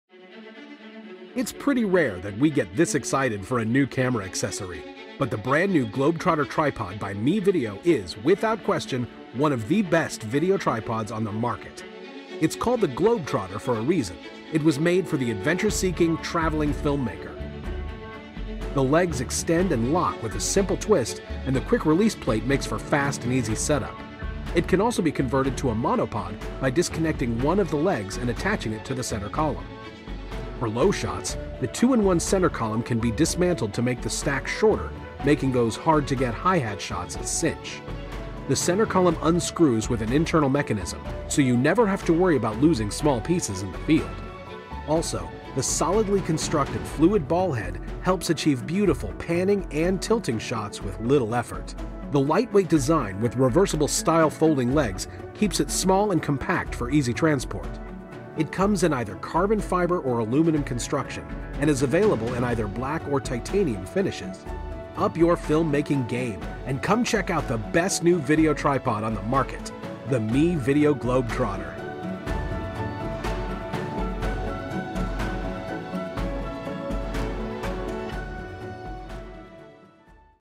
MeVIDEO Globetrotter Product Feature - NEW VIDEO TRIPOD
We put the new MeVIDEO Globetrotter video tripod through its paces.